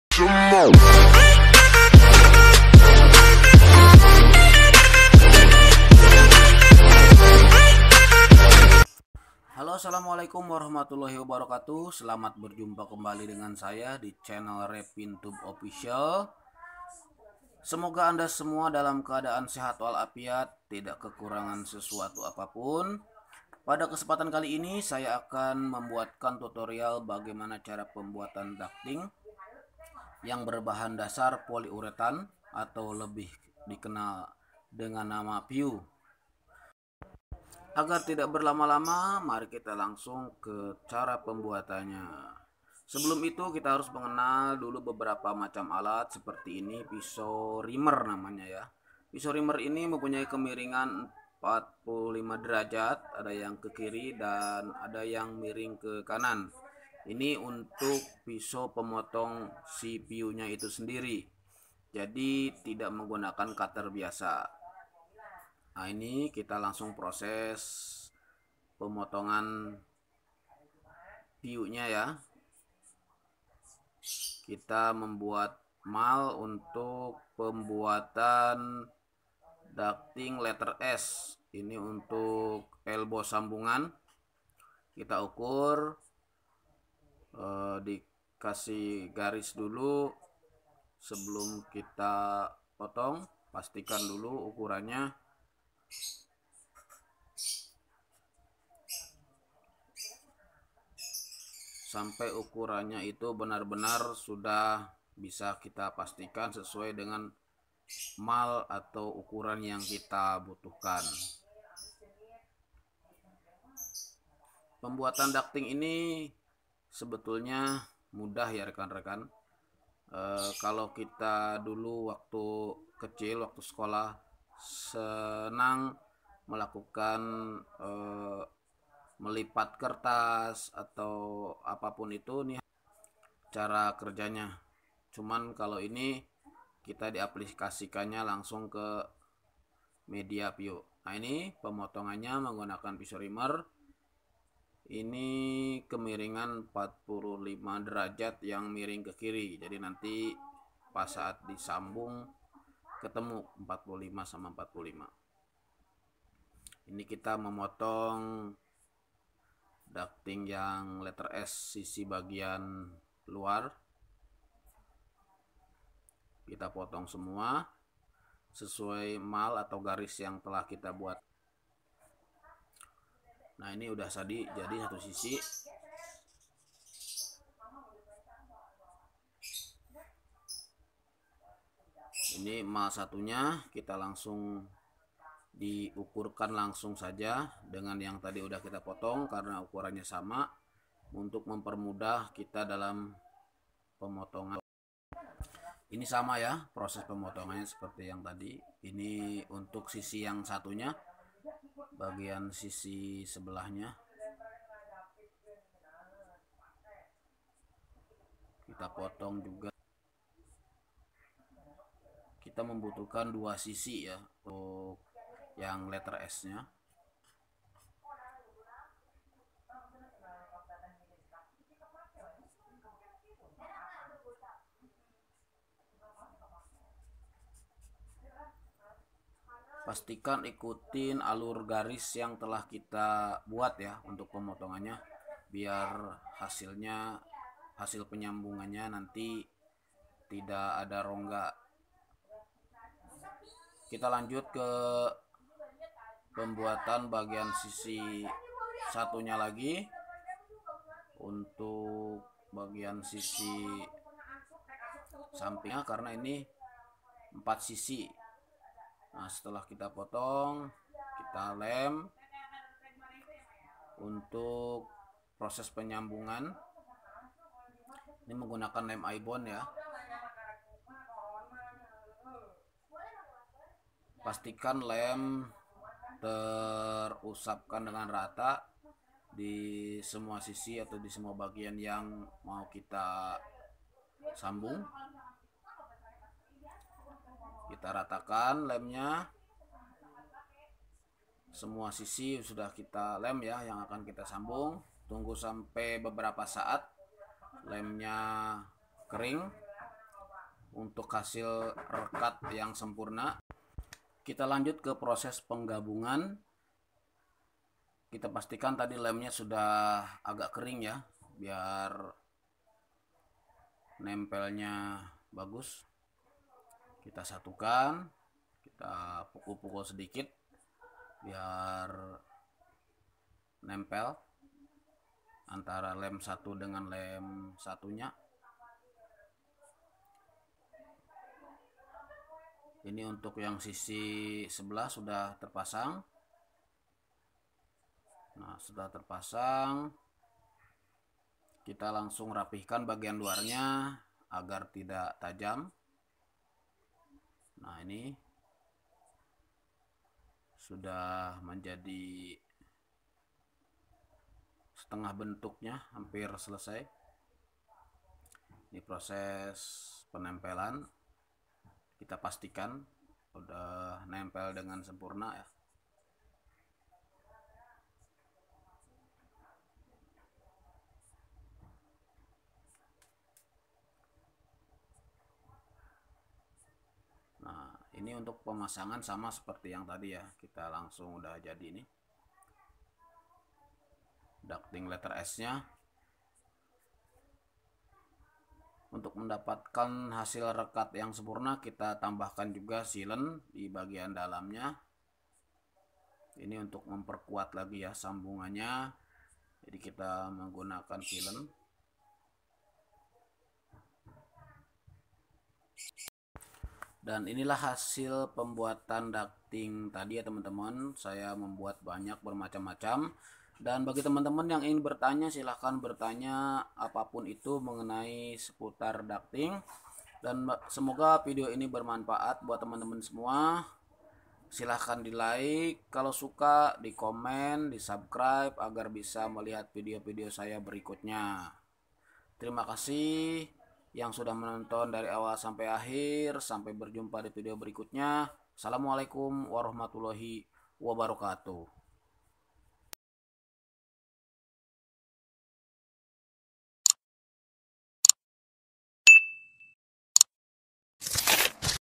Tutorial Cara mudah Membuat ducting PU #1
ducting ini terbuat dari poly urethan atu lebih dikenal dengan sebutan PU.
adapun perlengkapan lainya
lem aibon dan pisau reamer